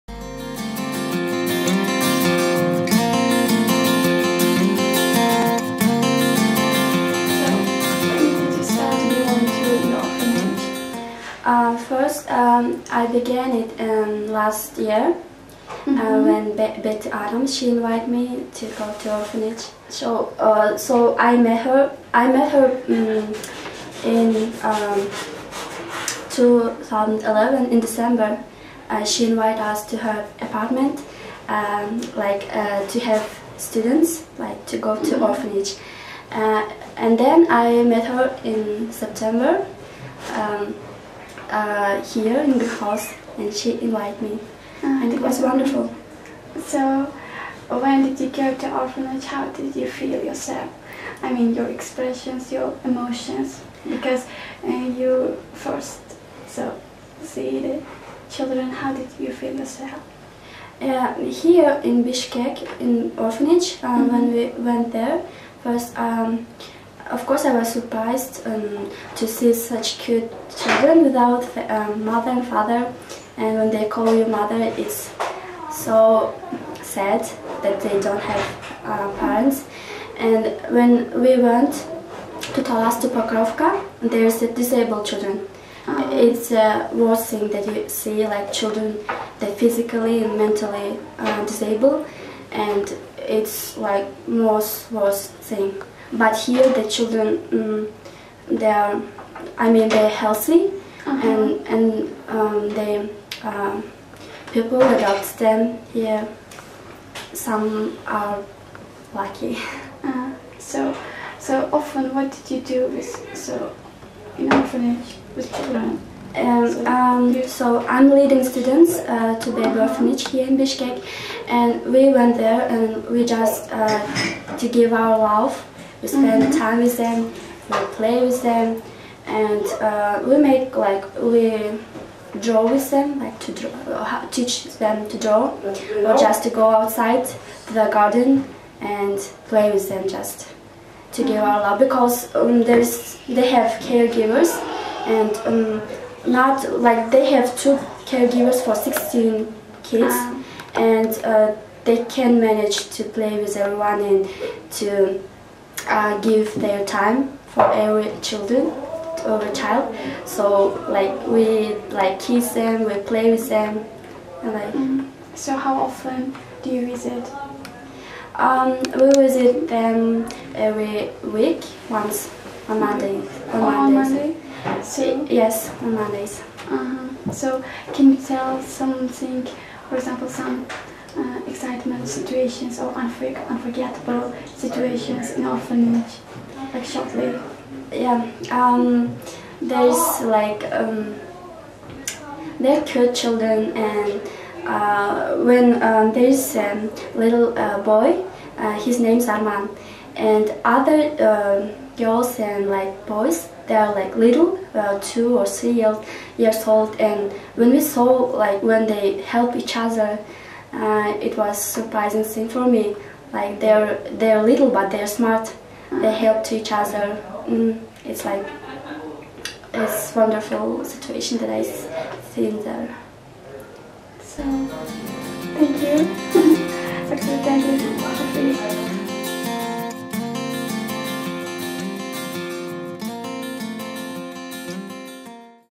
Perizod interviews Jyldyz Ormonbekova
Jyldyz is a volunteer with the Orphans Spirit of Salam projects and is a student at BHU.  In this interview she tells about her experience as a volunteer.